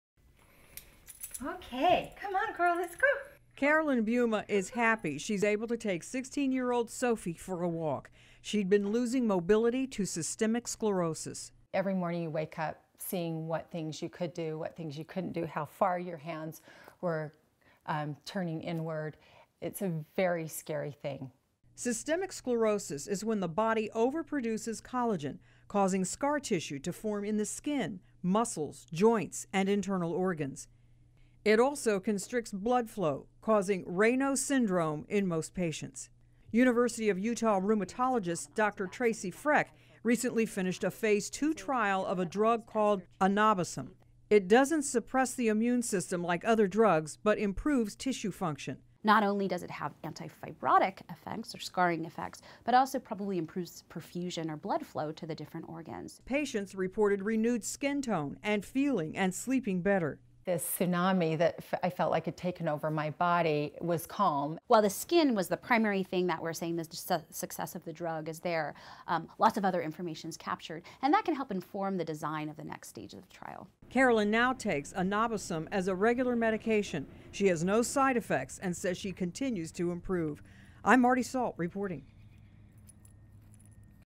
NEW HOPE FOR SYSTEMIC SCLEROSIS
Imagine your skin and your internal organs slowly thickening and hardening and there’s no cure. Results from a new drug study are promising for people suffering with systemic sclerosis